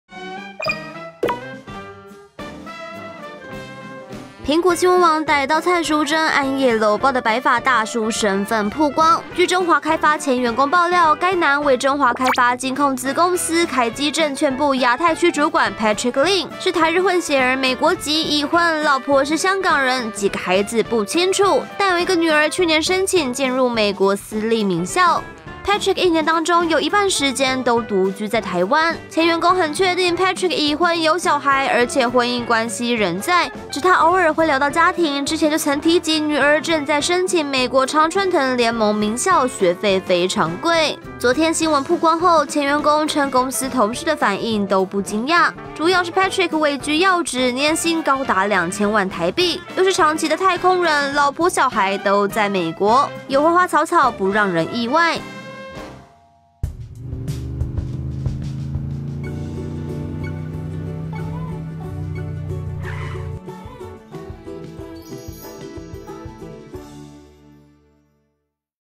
【獨家起底】蔡淑臻夜摟白髮大叔是年薪2千萬凱基證券高層！被爆已婚有女兒 | 蘋果娛樂 | 蘋果新聞網
《蘋果新聞網》逮到蔡淑臻暗夜摟抱的白髮大叔身分曝光，據中華開發前員工爆料，該男為中華開發金控子公司凱基證券部亞太區主管Patrick Lin（林俊成），是台日混血兒，美國籍，已婚，老婆是香港人，幾個孩子不清楚，但有一個女兒去年申請進入美國私立名校，Patrick一年當中有一半時間都獨居在台灣。對此，蔡淑臻表示知道對方有結婚，但是因為他們就是普通朋友。摟抱的部分因為對方就是一個外國人連中文都不會說，他們都是用英語交談，只是基本禮節而已；凱基則表示不回應個人私事部分。